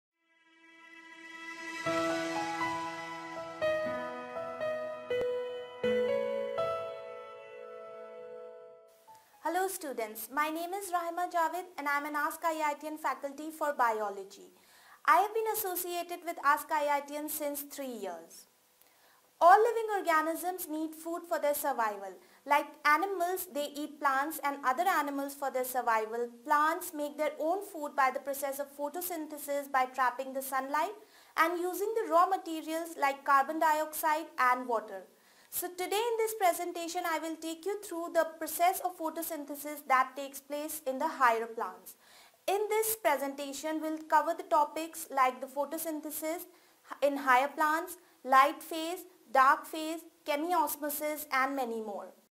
Photosynthesis in Higher Plants | Biology | Class 11 | NEET | AIIMS | askIITians
"Raheema Javeed explains in detail the Photosynthesis in Higher Plants as expected for NEET (AIPMT)/Medical Exams. Her lectures are extremely rich and help students in knowing the areas that require special attention. Watch the video to know the various ways of acing the AIPMT/Medical exams with ease.

This chapter covers Photosynthetic Pigments, Light Phase, Cyclic and Noncyclic Photophosphorylation, Chemiosmosis, CAM Plants, C4 cycle , C2 Cycle Or Glycolate Pathway, Calvin Cycle, Factors Affecting Photosynthesis"